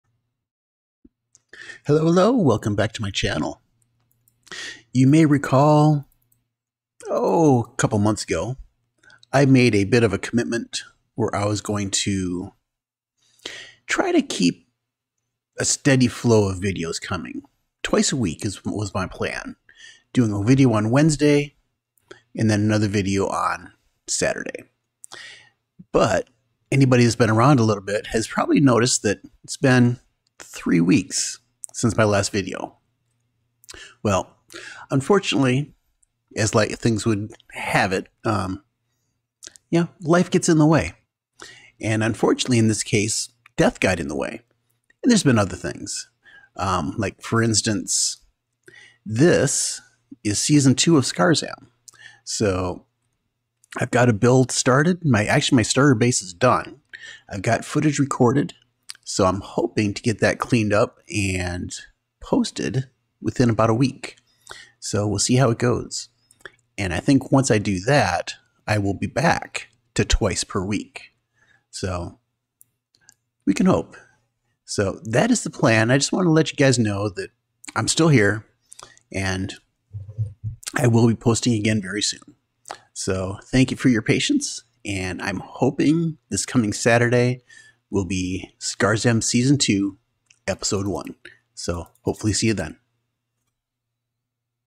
Vulpecula, where are you? The world wonders.
After promising to get two videos out per week, I did OK for about a month, and then Life (and death) got in the way. Then, season 1 ended, and there just wasn't anything to post.